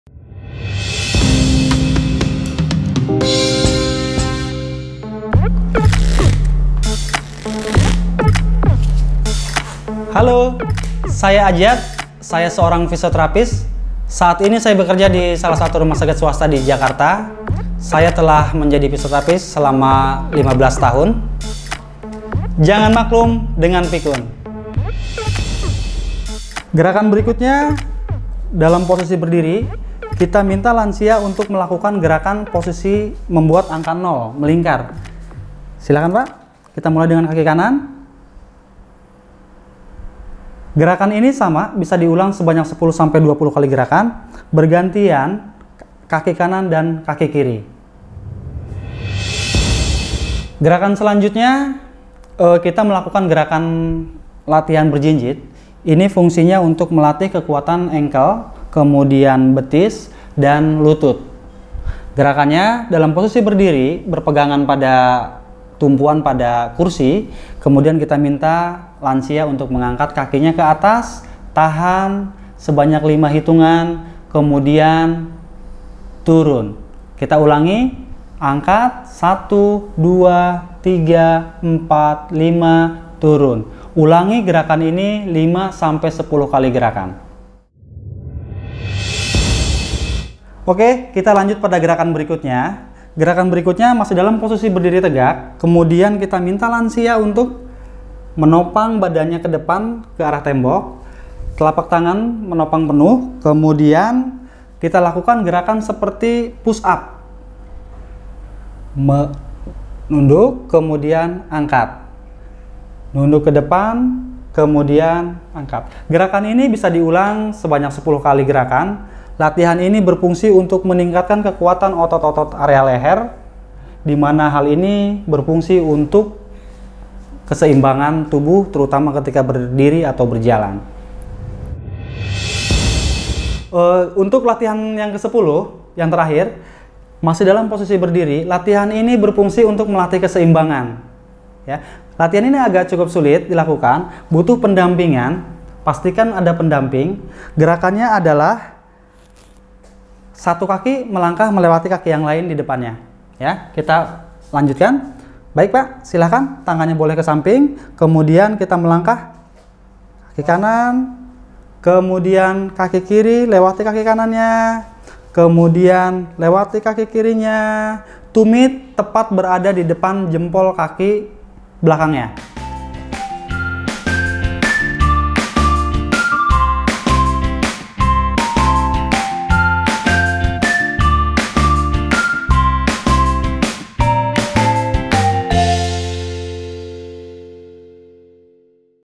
Latihan Fisioterapi (Bagian 2) oleh Ajat Sudrajat, S.FT
10 Gerakan Mengurangi Risiko Jatuh (Bagian Kedua)
Latihan 7: Dalam keadaan berdiri, membuat angka nol melingkar dengan kaki satu, secara bergantian kaki kanan dan kiri. Ulangi sebanyak 10-12 gerakan secara bergantian kaki kanan dan kiri.
Latihan 8: Latihan berjinjit,dalam posisi berdiri tegak dan bertumpu/berpegangan pada kursi, berjinjit angkat dan tahan selama 5 hitungan lalu turun. Ulangi sebanyak  5-10 kali gerakan.
Latihan 9: Masih dalam posisi berdiri, menopang badan  ke arah tembok, telapak tangan menapak penuh, lakukan gerakan seperti push up, diulang 10 kali gerakan.
Latihan 10: Masih dalam posisi berdiri, gerakannya adalah melangkah satu kaki melewati kaki yang lain di depannya. Dua tangan boleh ke samping.

Ajat Sudrajat, S.FT
Fisioterapis